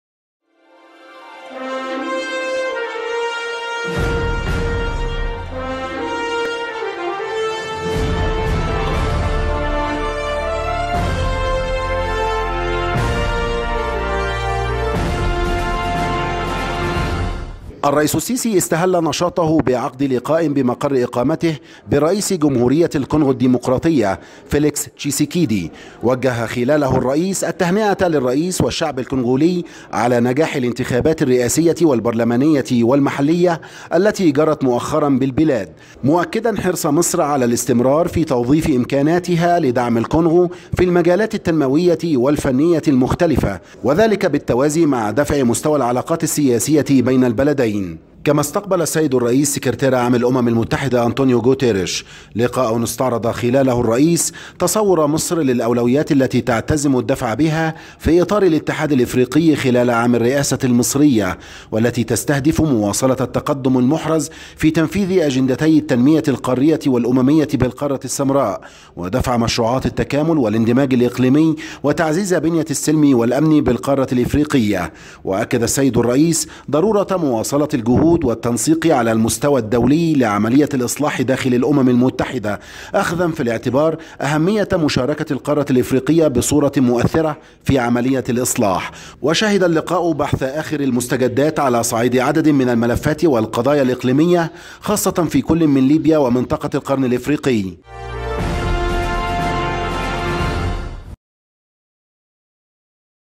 تفاصيل لقاء السيسي مع الأمين العام للأمم المتحدة في أديس أبابا
التقى الرئيس عبدالفتاح السيسي، اليوم، مع أنطونيو جوتيريش، سكرتير عام الأمم المتحدة، بمقر إقامته بأديس أبابا، حيث أكد الرئيس تطلع مصر لأن تشهد فترة الرئاسة المصرية للاتحاد الأفريقي نقلة نوعية في اتجاه تعزيز وتعميق الشراكة الاستراتيجية بين الاتحاد والأمم المتحدة، وذلك تحت مظلة الأطر القائمة للتعاون بين المنظمتين، لاسيما في مجالي السلم والأمن والتنمية
تابعوا قناة Ten TV على مواقع التواصل الاجتماعي: